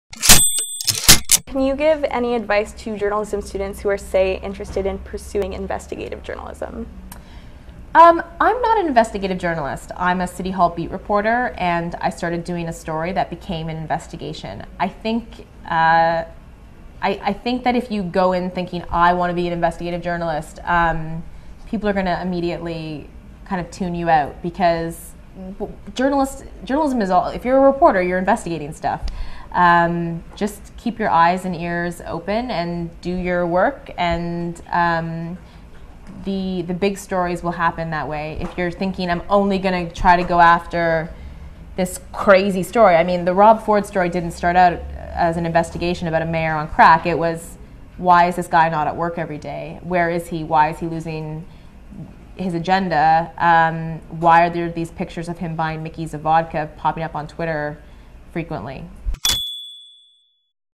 Robyn Doolittle's advice for journalism students
Robyn Doolittle explains the importance of having investigative skills as a journalist, no matter what beat you're covering,